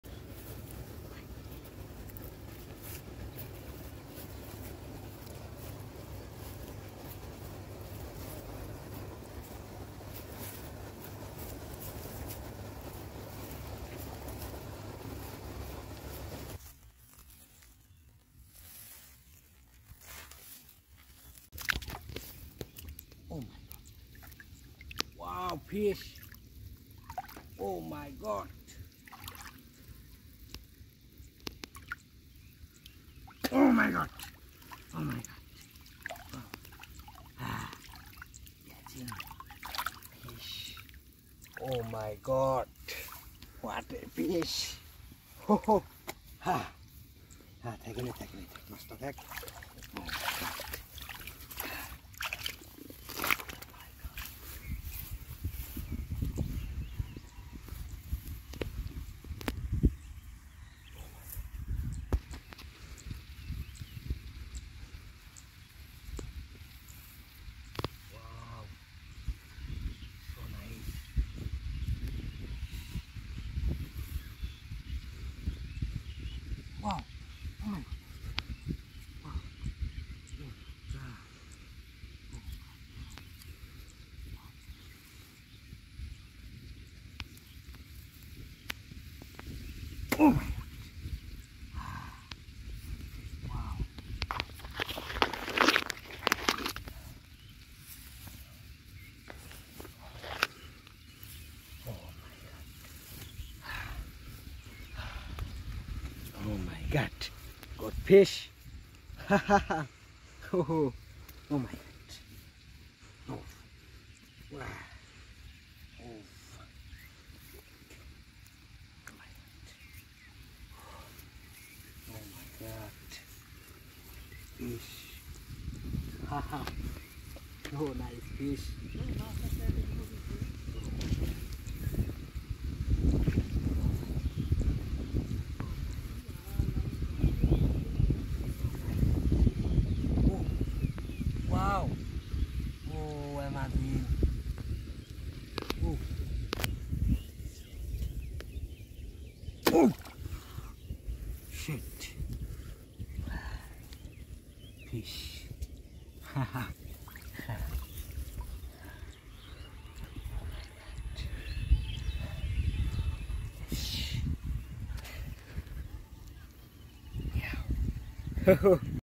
Amazing Sight Of Tilapia Fishing With Bow🥰A Boy Catching Wonderful Tilapia Fish By Arrow
🌸Cameraman and Director🌸

@ Md Habib Sikder 🌚🦋🥀

Actor🎬